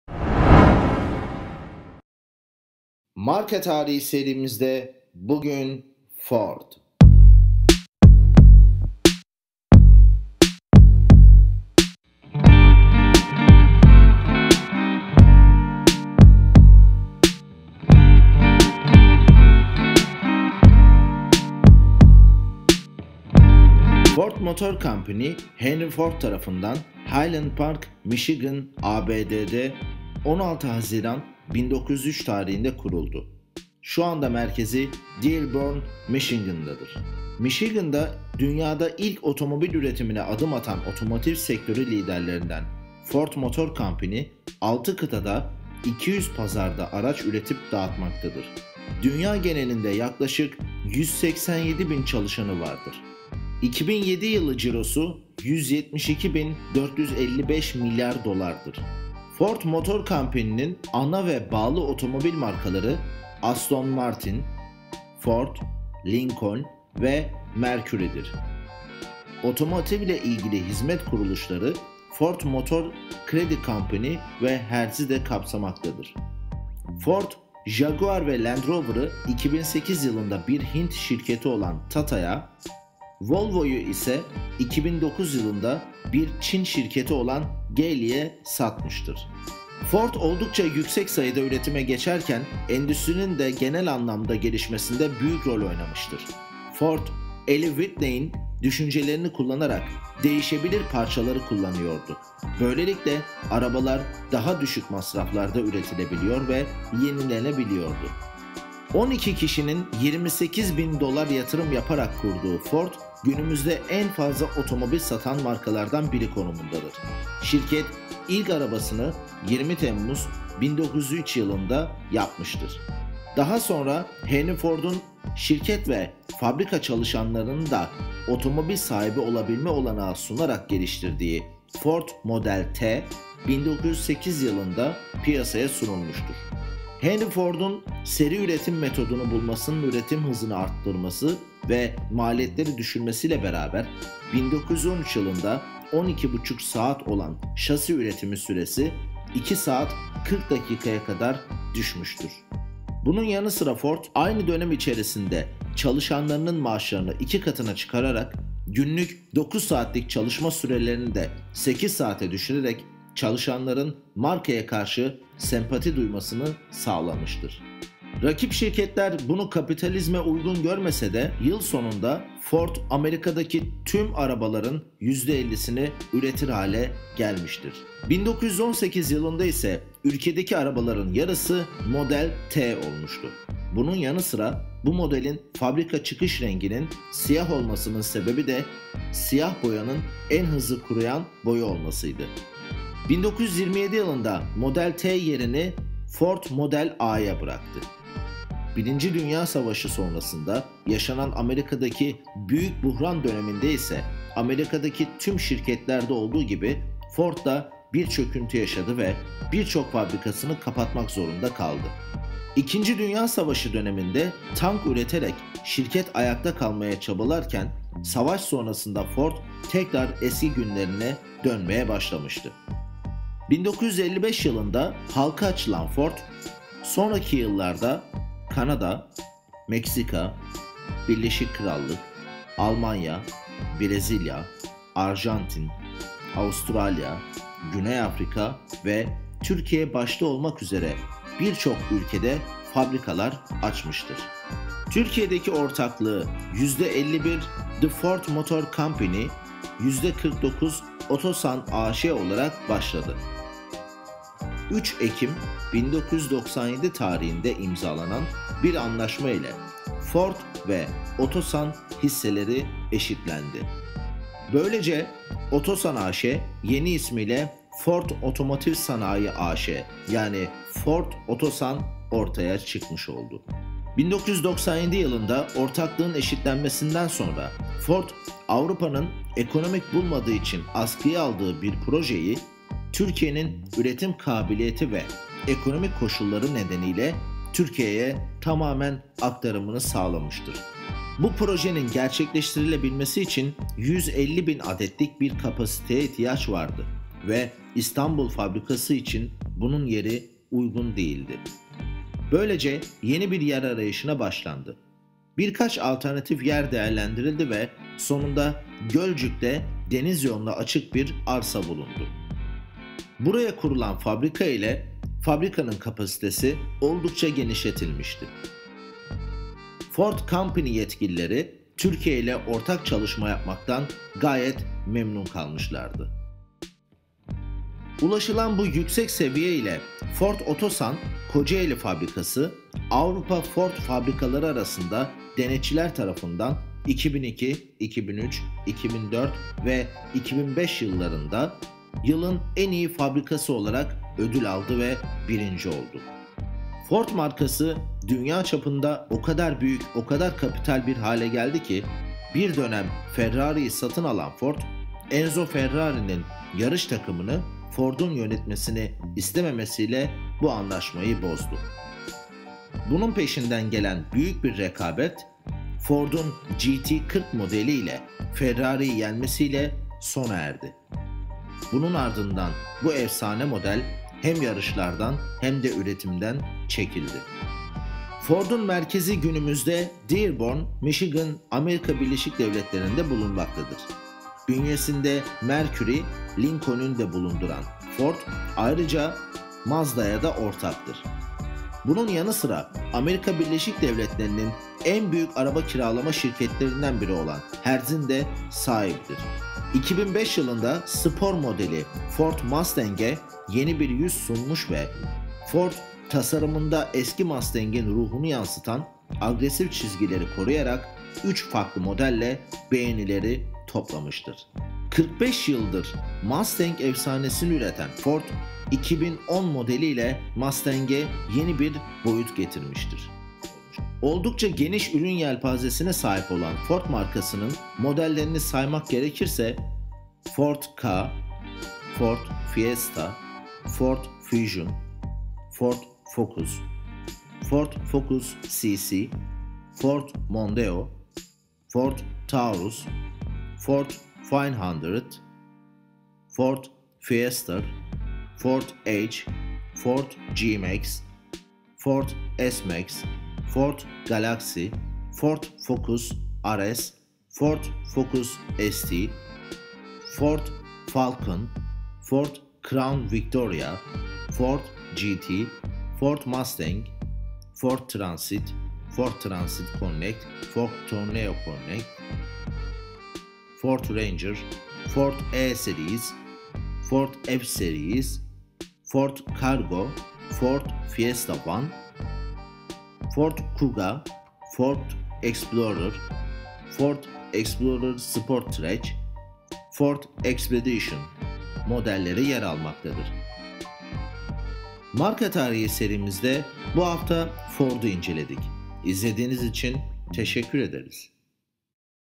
FORD MARKA TARİHİ | Ranger Mı? X Mi? | Marka Tarihi Serisi
Marka Tarihi serimizin, bu altıncı bölümünde FORD markasını yakından inceledik ve markanın bilinmeyen yönlerini sizler için bir araya getirdik.

     Fikirlerinizi ve incelememizi istediğiniz farklı markaları yorum kısmına yazabilirsiniz.

     İyi seyirler!